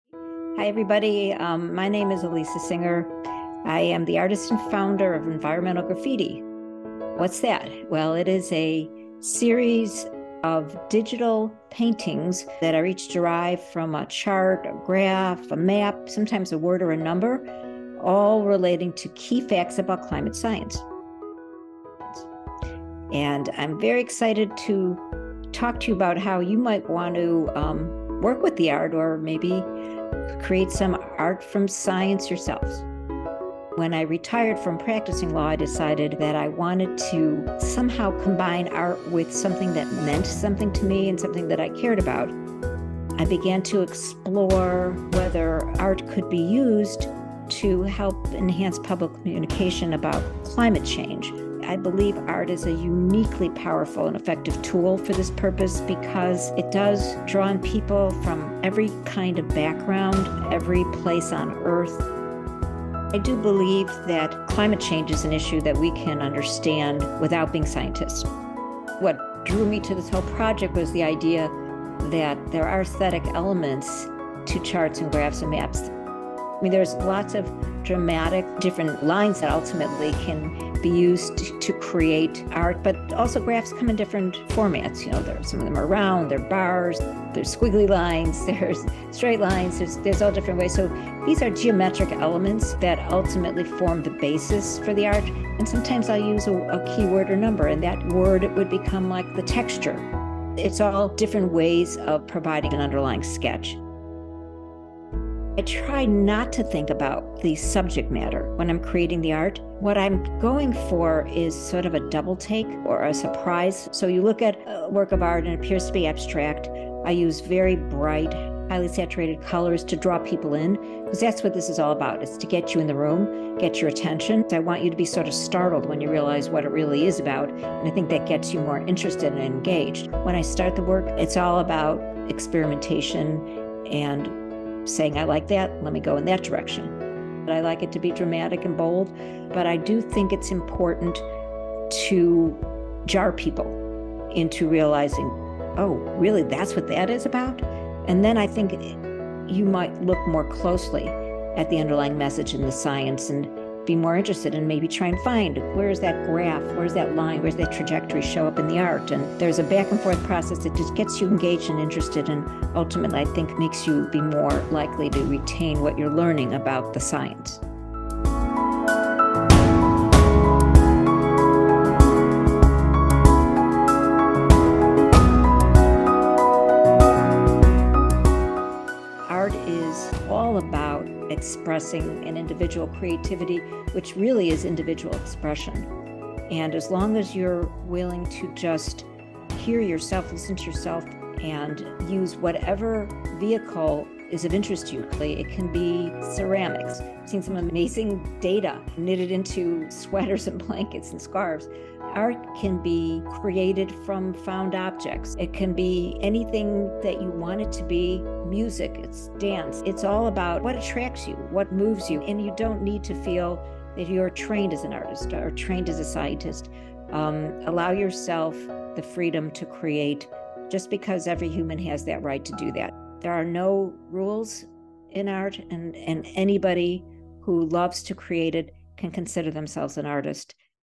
Where to Start with Science Art
Learn about science and art from professional artist Alisa Singer, who believes climate change to be the most critical challenge facing our world and sought to find a way to use her art to further efforts to bring awareness to the issue. She was attracted by the inherently aesthetic design elements of scientific charts and graphs, and intrigued by the idea of using art to give them dramatic effect. We've teamed up to design an activity for students of all ages to draw inspiration from scientific figures to create and share original art pieces!

Video produced by AJ Mallozzi and Alyssa Pratt